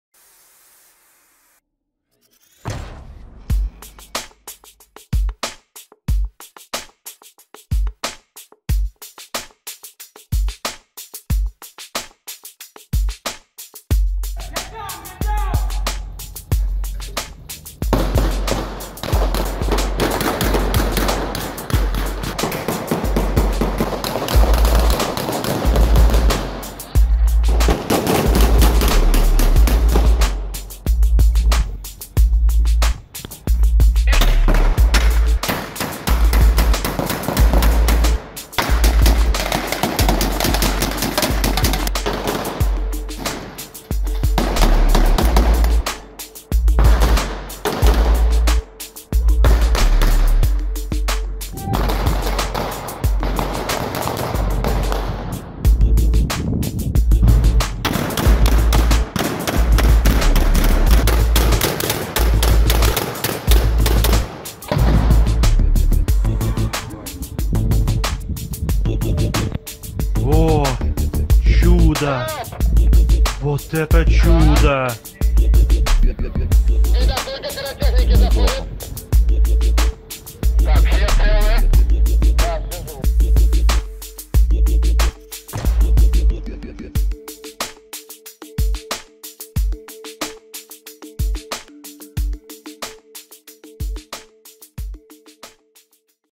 Shooting in Old Riga.Roni & Glock 17--ReduT Film-- Name of Movie --Hero--- . Project 2018
Our blank gun's firing in Russia action movie HERO.
Movie shooting was in Riga, old city part.
Redut Film